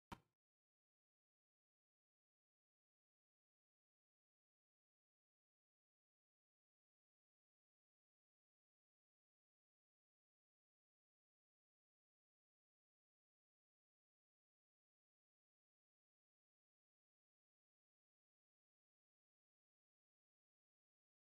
稲沢店 ミニチュアシュナウザー 294389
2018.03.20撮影
コロコロの体でよく遊びます。
すぐお腹をみせる甘えん坊です。